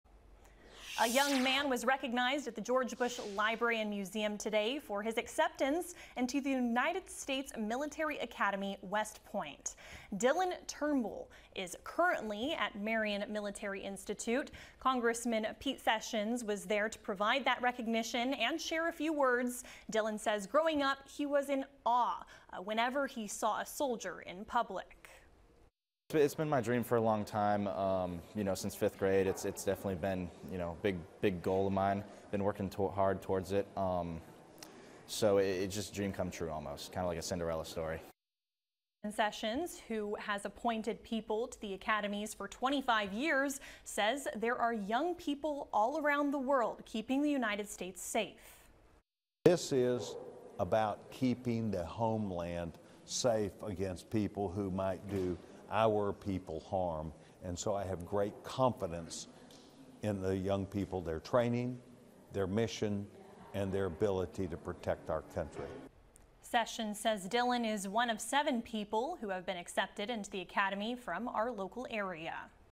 Student's acceptance to West Point celebrated
A student is celebrated for his acceptance to West Point.